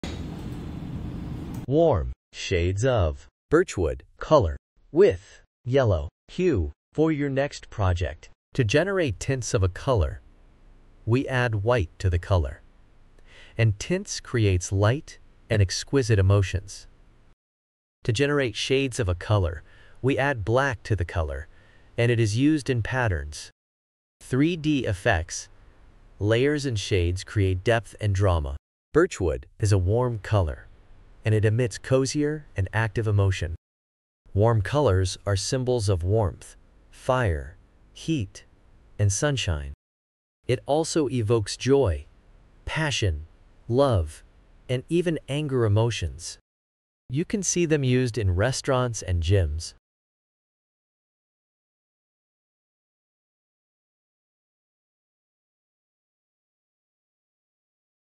Shades & Tints of Birchwood color ccbeac A Warm Yellow color
Shades of Birchwood color ccbeac Warm Shades of Birchwood color with Yellow hue for your project!

Shades
Hex b5a898

Tints
Hex d1c5b5

Birchwood is a warm color, and it emits cozier and active emotion. Warm colors are symbols of warmth, fire, heat, and sunshine. It also evokes joy, passion, love, and even anger emotions. You can see them used in Restaurants and Gyms.